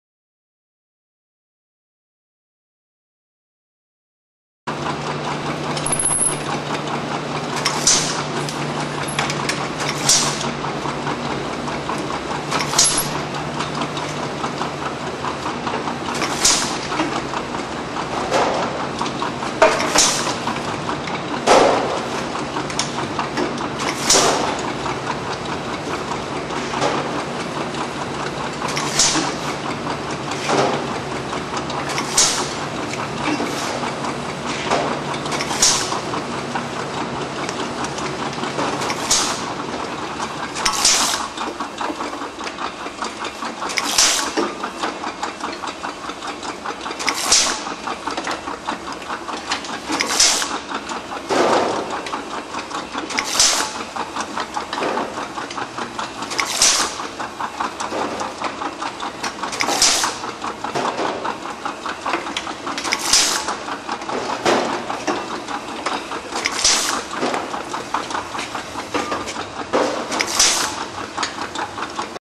Pneumatic Product Top Off System for Gylcol or Water ABA-M227
Anthony Barber Associates, Ltd.
Model: ABA-TOPOFF-N
Pneumatic Product Top Off System for Gylcol or Water
Suitable for ANY environment